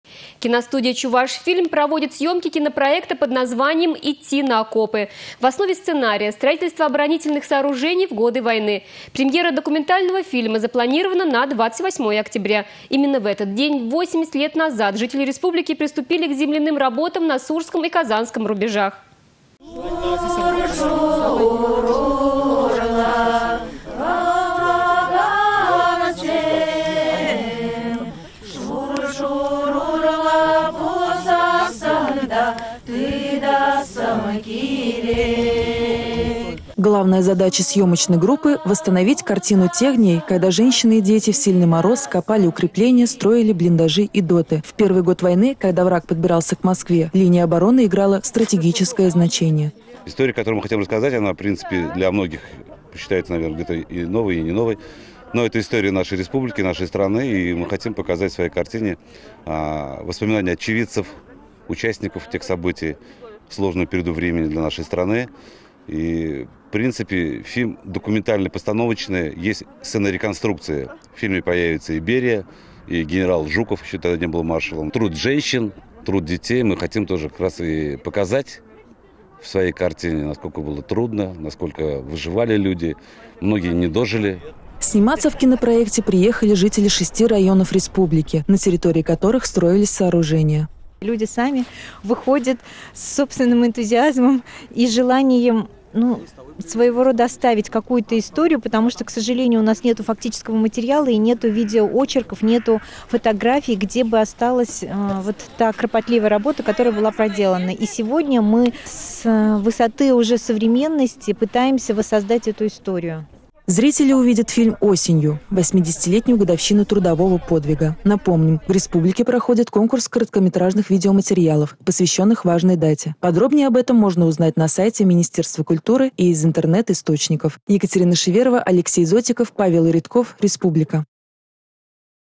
В Чувашии проходят съемки кинопроекта о строительстве оборонительных сооружений
Киностудия «Чувашфильм» проводит съемки кинопроекта под названием «Идти на окопы». В основе сценария — строительство  оборонительных сооружений в годы войны. Премьера документального фильма запланирована на 28 октября. Именно в этот день 80 лет назад жители республики приступили к  земляным работам на  Сурском и Казанском  рубежах.